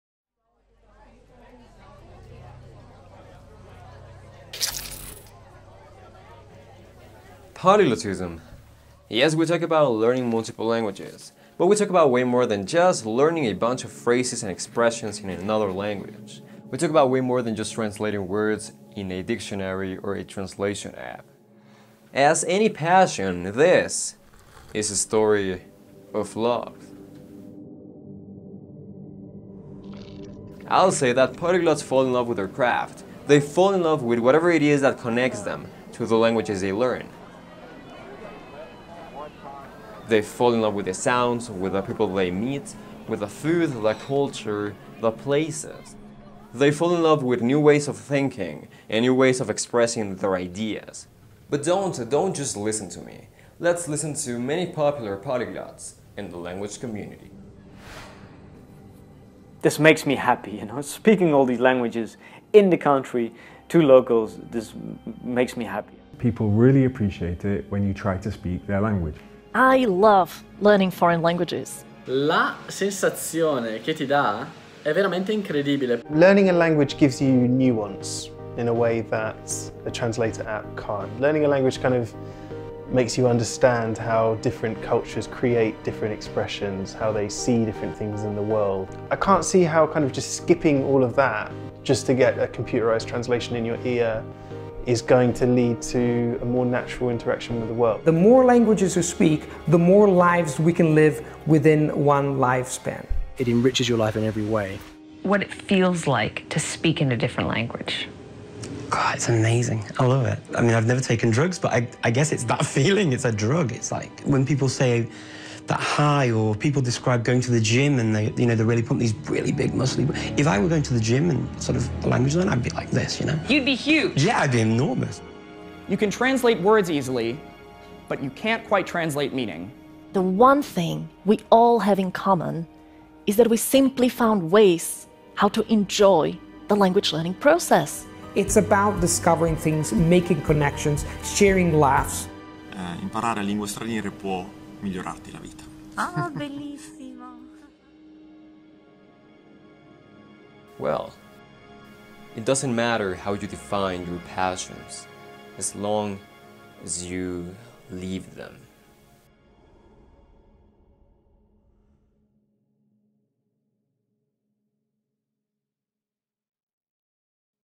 POLYGLOTISM | The Beauty of Languages
Polyglotism might be defined as the ability to speak many languages... but, the truth is that it's way more than just that! It's way more than just learning a bunch of phrases and expressions in another language, way more than just learning words; it's about a way of living, new ways of perceiving the world, new ways to express our ideas and much more!  | Let's listen together to what this amazing polyglots have to say!! :D

Feeling like sending me an e-mail?